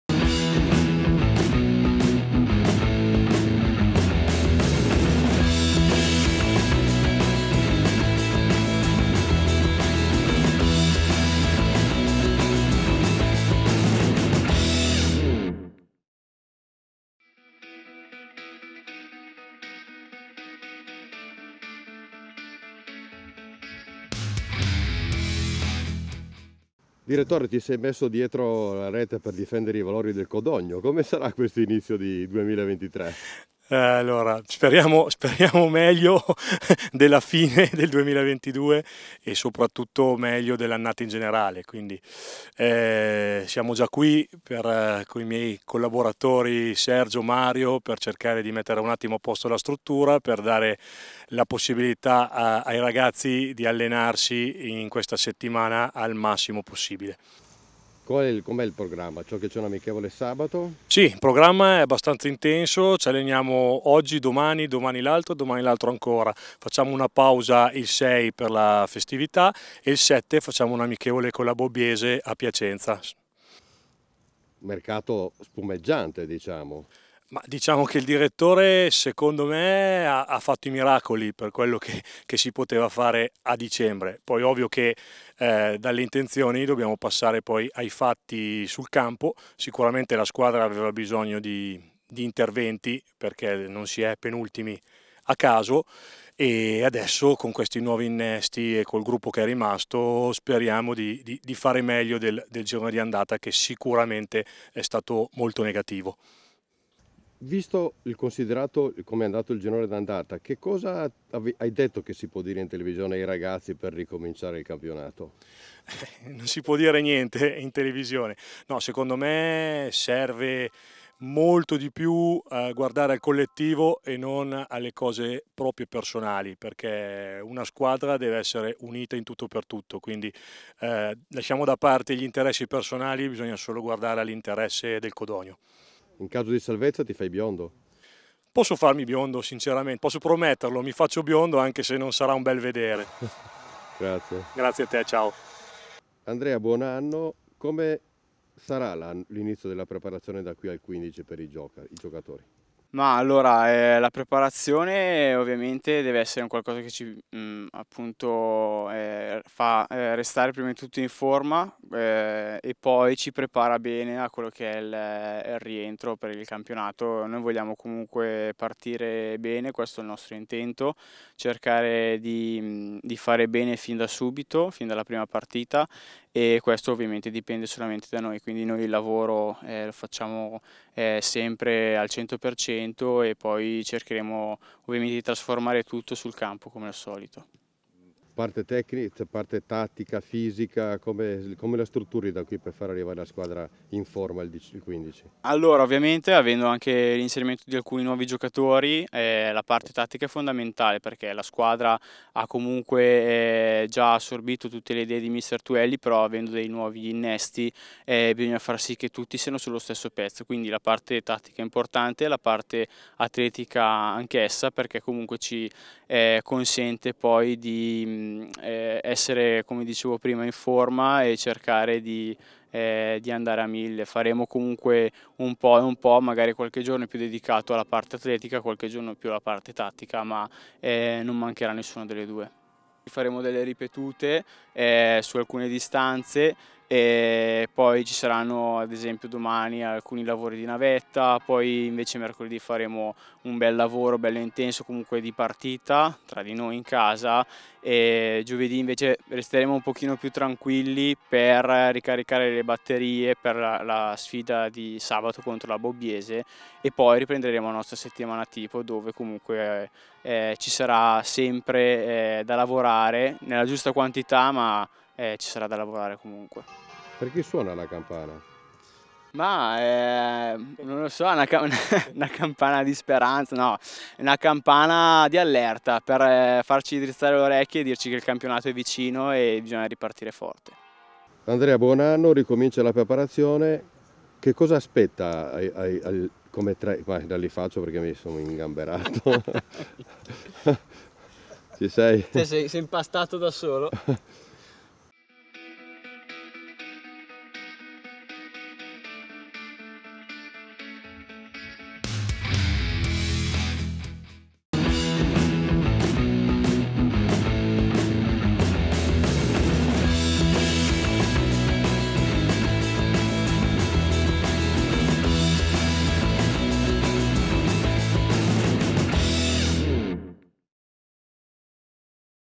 SPAZIO CODOGNO - RIPRESA PREPARAZIONE NUOVO ANNO - PRIMO ALLENAMENTO
Riprendono la preparazione i Lupi di Mister Lucchi Tuelli. Al microfono di Arizona Tv il Dir. tecnico organizzativo Michele Marseglia ed il preparatore atletico Andrea Pasquetto.